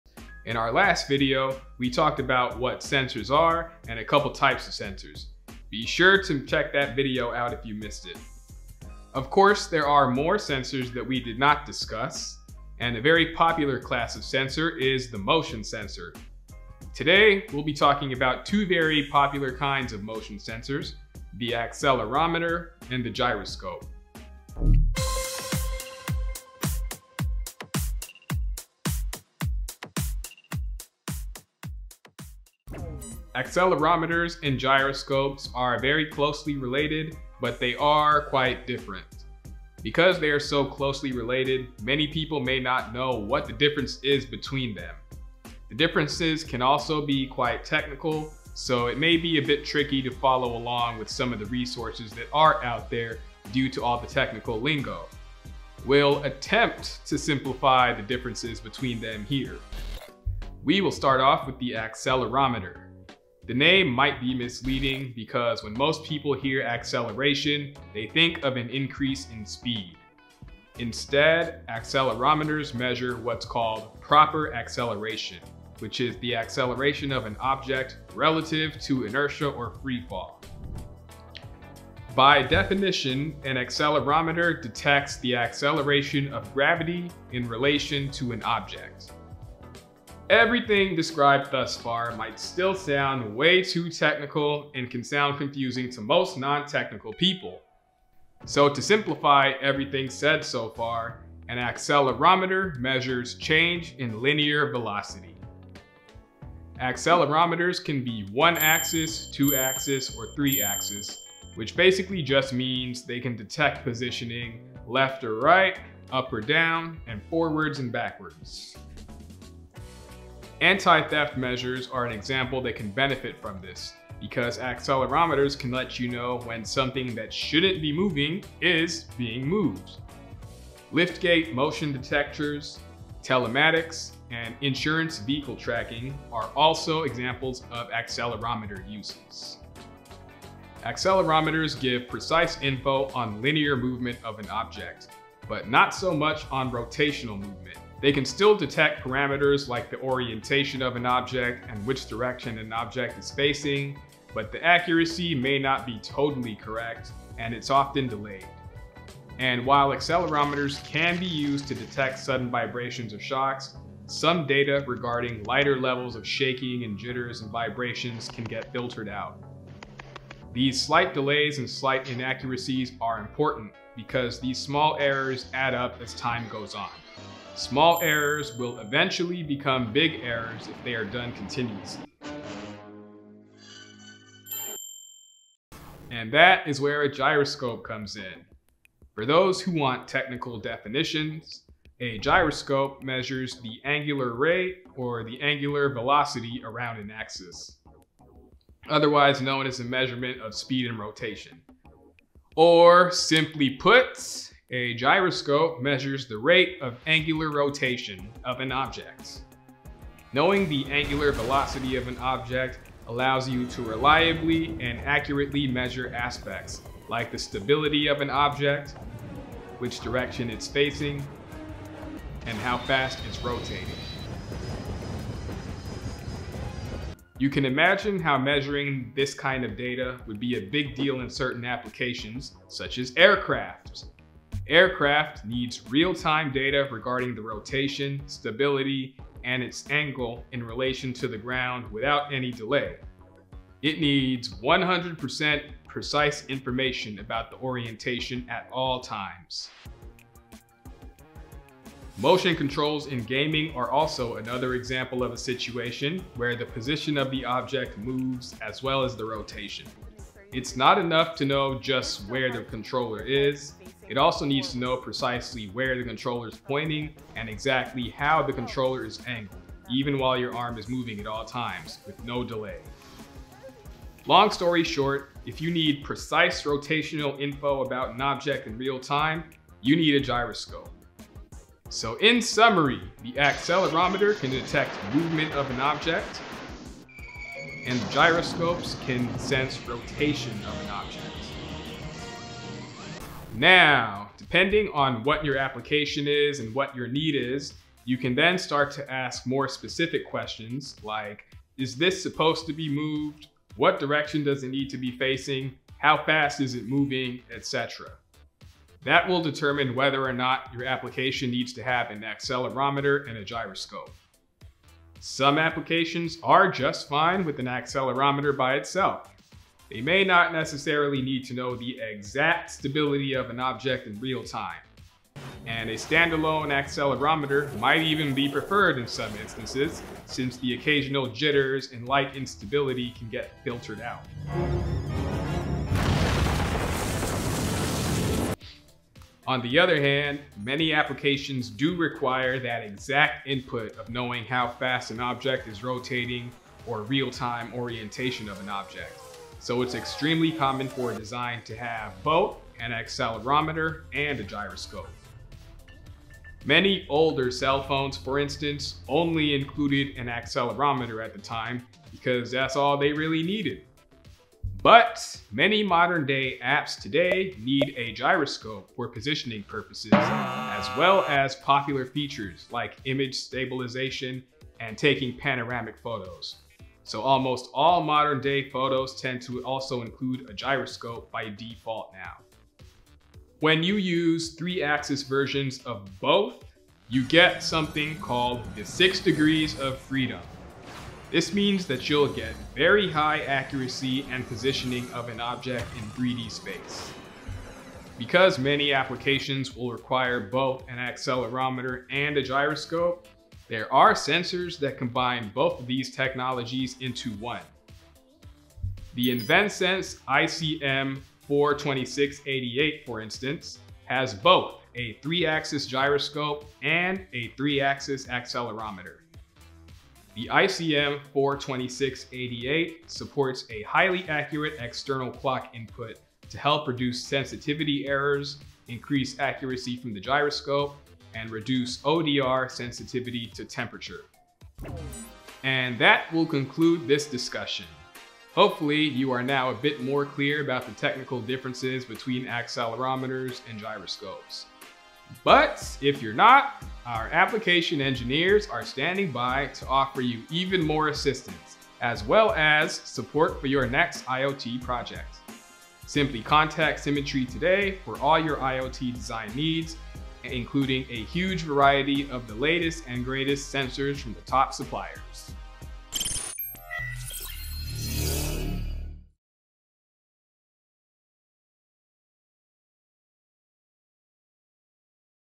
Accelerometer vs. Gyroscope - What's the Difference Between These Popular Sensors?
The motion sensor is a very popular class of sensor that sees frequent usage in IoT products. Two particular kinds of motion sensors that are common are accelerometers and gyroscopes. Both sensors are often included together in many devices, and many people aren’t sure of what the exact differences are between the two. In this video, Symmetry Electronics takes a swing at providing a bird’s eye overview of the differences between accelerometers and gyroscopes while trying to limit the technical details.

0:00 Intro
0:55 Accelerometers
3:05 Gyroscopes
4:52 Which One to Use?
7:14 Closing Words